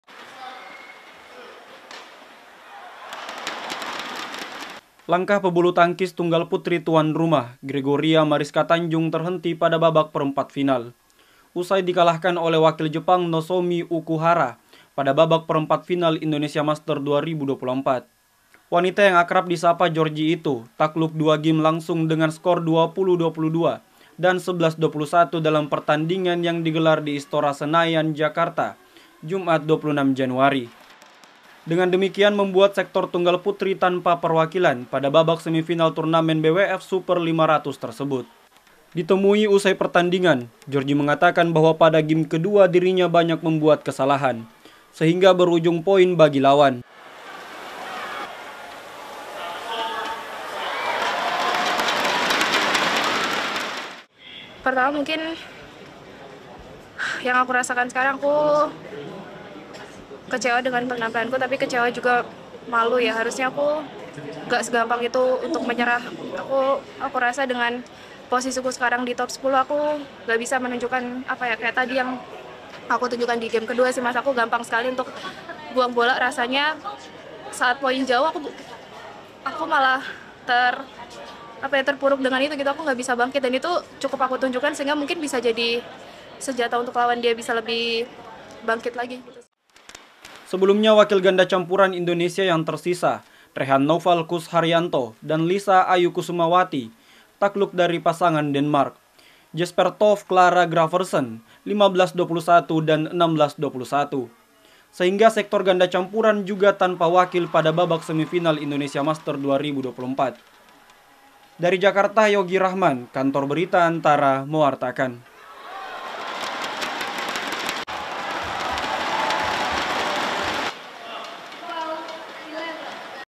Tunggal putri tanpa wakil di semifinal Indonesia Masters 2024
ANTARA - Pebulu tangkis tunggal putri tuan rumah Gregoria Mariska Tunjung kalah dari wakil Jepang Nozomi Okuhara pada laga perempat final Indonesia Masters 2024 dengan skor 20 - 22 dan 11 - 21. Hal tersebut membuat sektor tunggal putri tanpa perwakilan di semifinal Indonesia Masters 2024. (Yogi Rachman/Rayyan/Gracia Simanjuntak/Ange Tabitha)